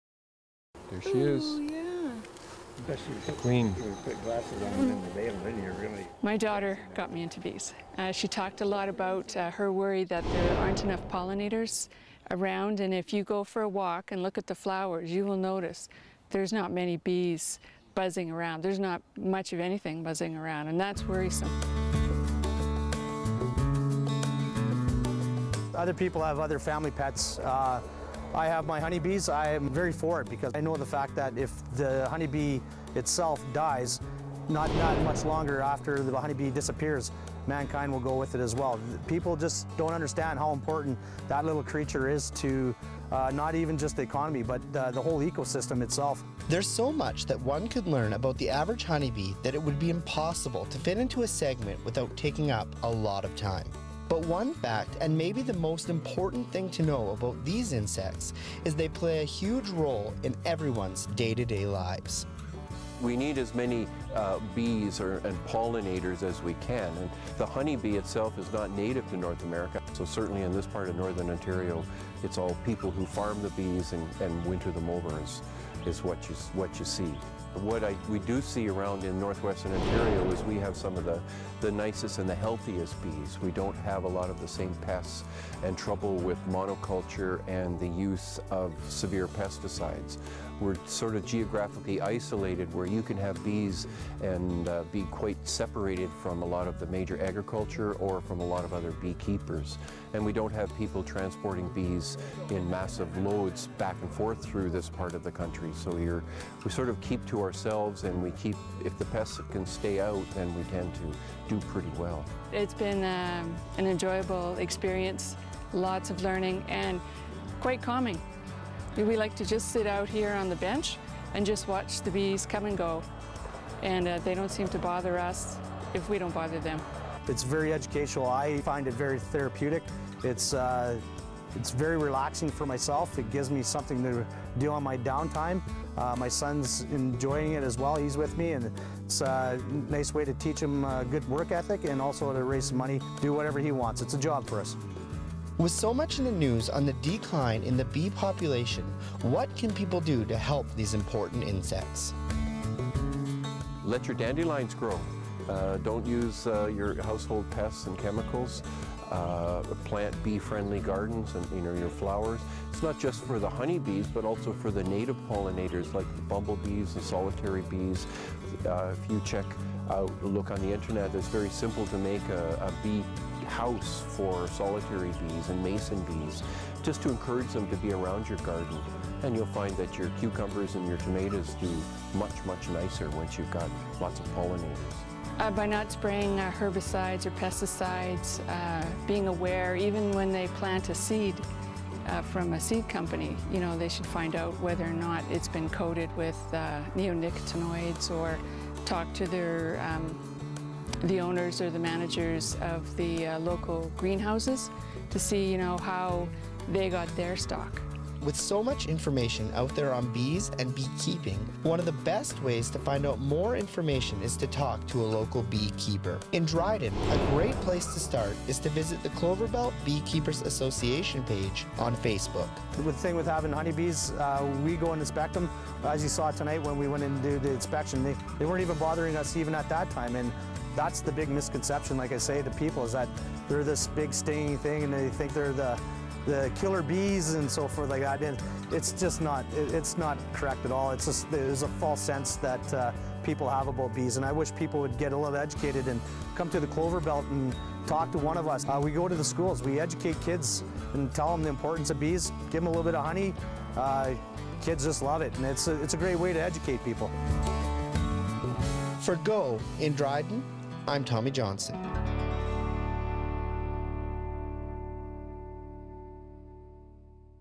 Cloverbelt Beekeepers Association
A look at local bee keepers in Dryden and surrounding area and the Cloverbelt Beekeepers association